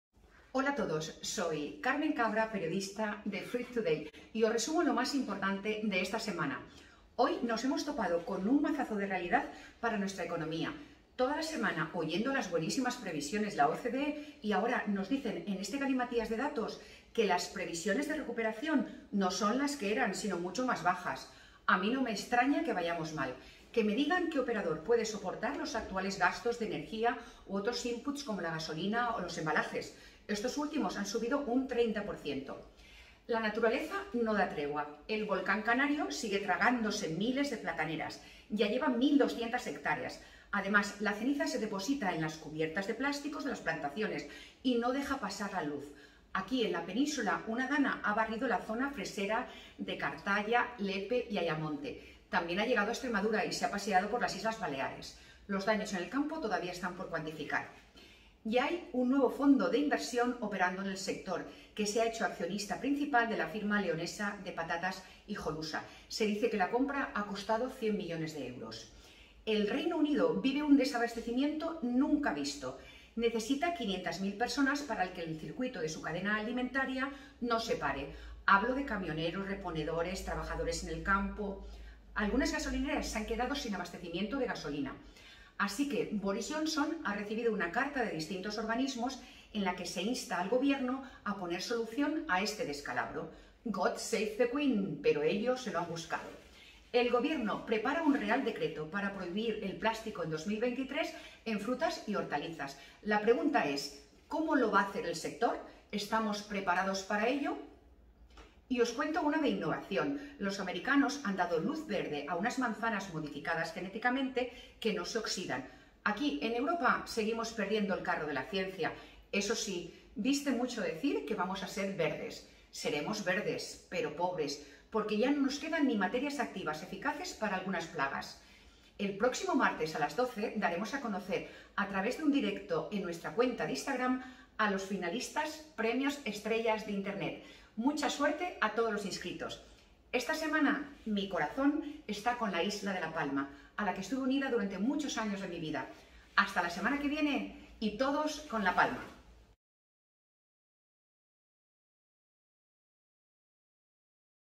LAS NOTICIAS A MI MANERA (semana 20 a 24 sept.)
Para aquellos a los que la vida no os da para más, pero queréis estar informados y disfrutar de vuestras familias y otros placeres, Carmen Cabra, directora de Fruit Today, resume lo más importante de la semana en el sector agrícola.